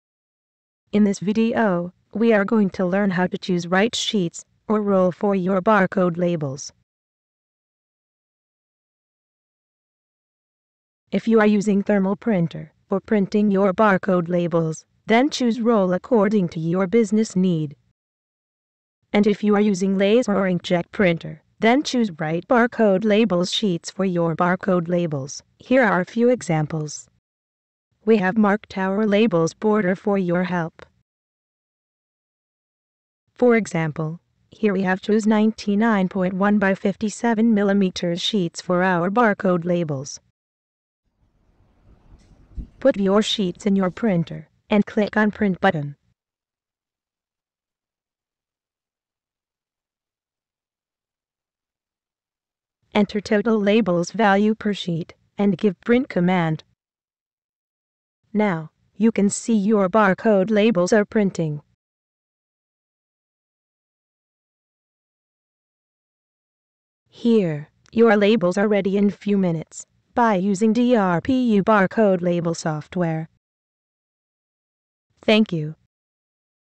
Selection of Rolls and Sheets for Barcode Stickers Printing
In this brief demo video you will see Selection of appropriate Rolls and Sheets for barcode stickers printing. You can create barcode stickers in different dimension accordingly by using DRPU Barcode Generator Application.